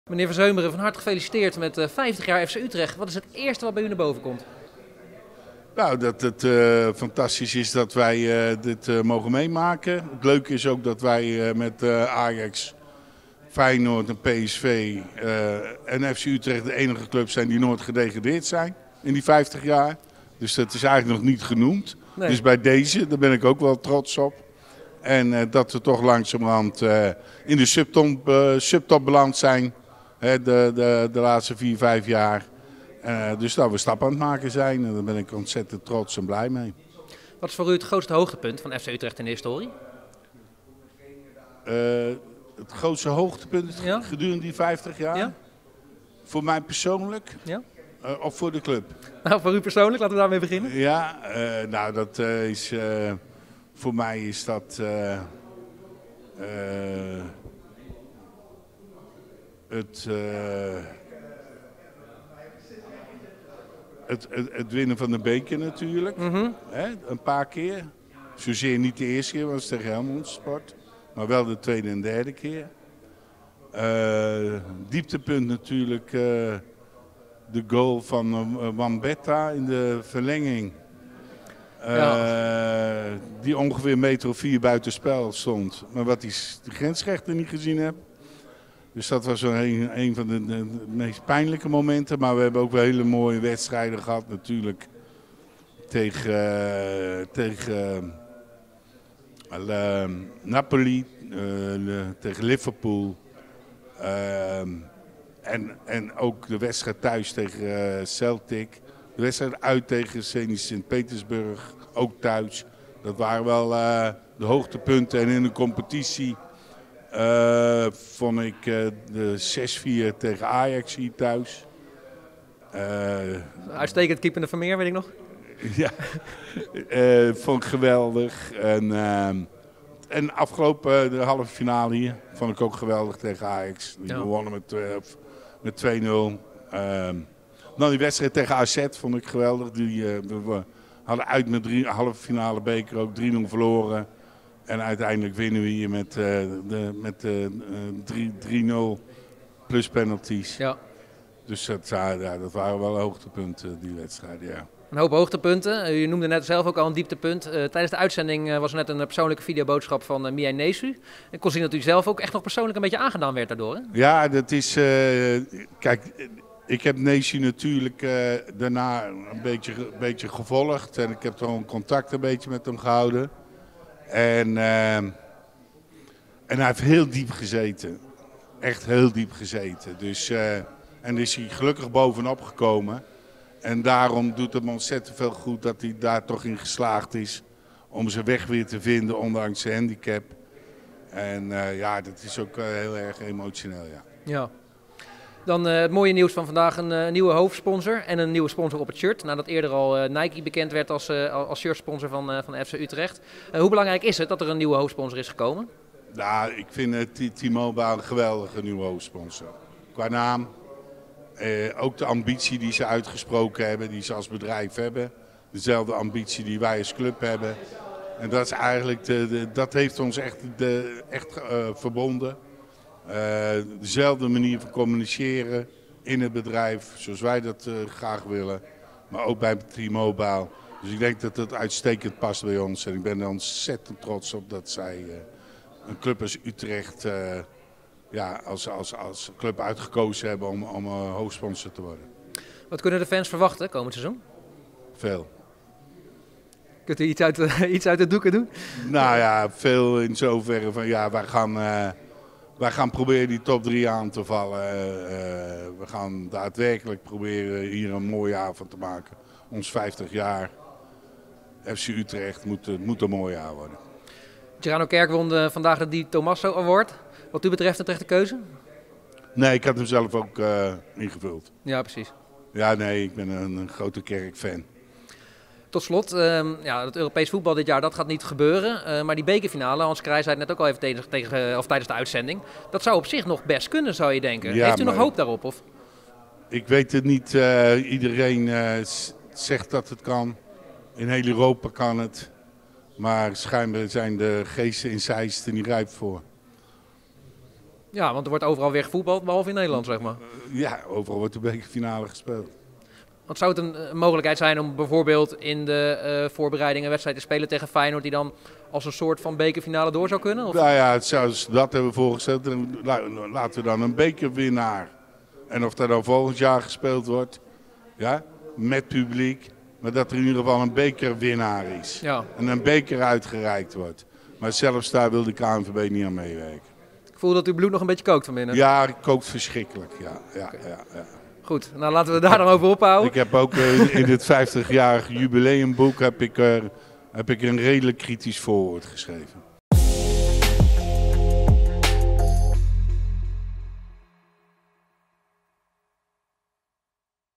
Van Seumeren over bekerfinale: 'Ik kook nog steeds verschrikkelijk!'
FC Utrecht bestaat vijftig jaar en dat werd gevierd met een live talkshow vanuit Stadion Galgenwaard met onder meer Kees Jansma, Hans Kraay jr., Frans van Seumeren en Willem Janssen. Na afloop verschenen deze vier heren voor de camera van Voetbalzone en directeur Frans van Seumeren blikt in gesprek met verslaggever Justus Dingemanse terug op vijftig jaar FC Utrecht en keek vooruit naar de toekomst.